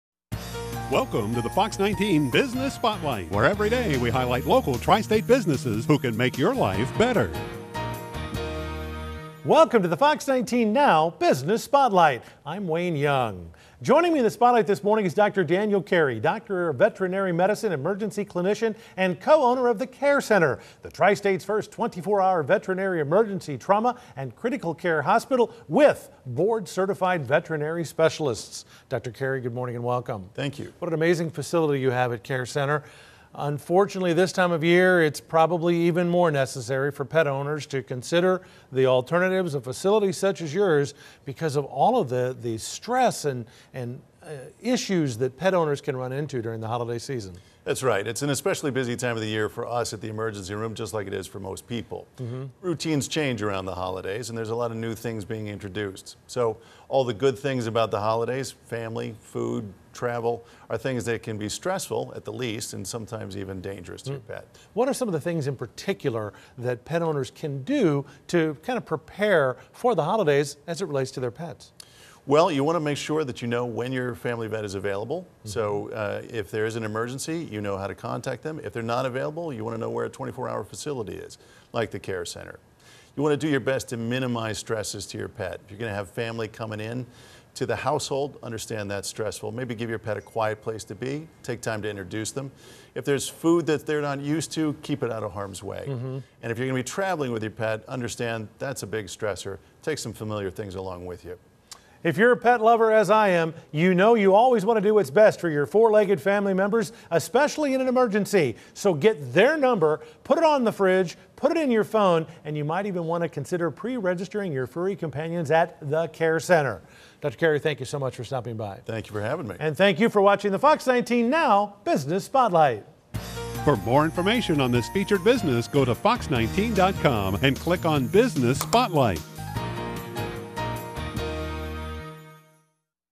FOX19 Now Business Spotlight: Care Center - Holiday Harm to Pets
Dr. Daniel Carey of the Care Center in Cincinnati warns pet owners that the holidays can be a stressful and potentially dangerous time for your pets.